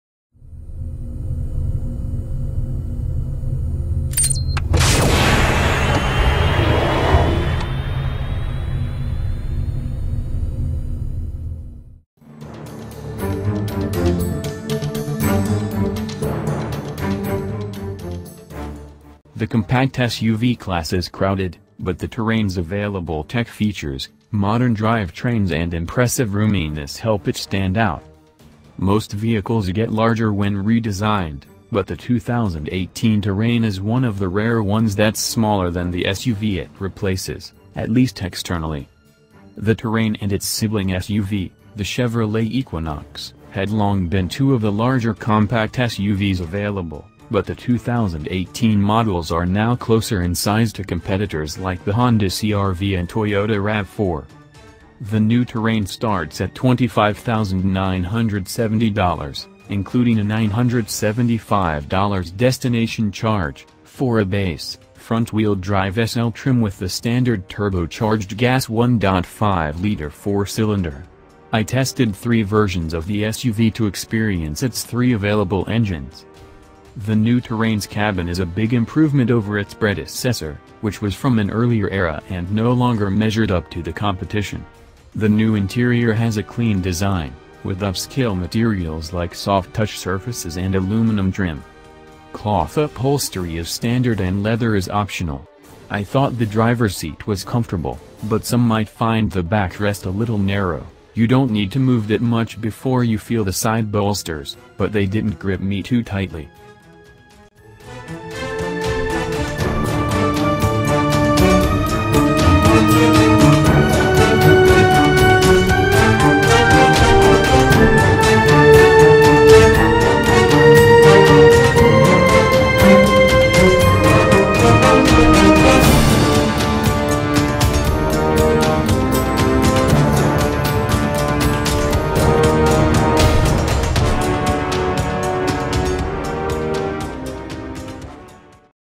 [Watch Now] 2018 GMC Terrain Price - Compact SUV Starts at $25,970 in U.S
2018 GMC Terrain Price - Compact SUV Starts at $25,970 in U.S

The compact SUV class is crowded, but the Terrain's available tech features, modern drivetrains and impressive roominess help it stand out.

Most vehicles get larger when redesigned, but the 2018 Terrain is one of the rare ones that's smaller than the SUV it replaces — at least externally.

The Terrain and its sibling SUV, the Chevrolet Equinox, had long been two of the larger compact SUVs available, but the 2018 models are now closer in size to competitors like the Honda CR-V and Toyota RAV4

The new Terrain starts at $25,970, including a $975 destination charge, for a base, front-wheel-drive SL trim with the standard turbocharged gas 1.5-liter four-cylinder. I tested three versions of the SUV to experience its three available engines:

The new Terrain's cabin is a big improvement over its predecessor, which was from an earlier era and no longer measured up to the competition. The new interior has a clean design, with upscale materials like soft-touch surfaces and aluminum trim.

Cloth upholstery is standard and leather is optional. I thought the driver's seat was comfortable, but some might find the backrest a little narrow; you don't need to move that much before you feel the side bolsters, but they didn't grip me too tightly.

Luck my other vidio.

2018 GMC Terrain Price - Compact SUV Starts at $25,970 in U.S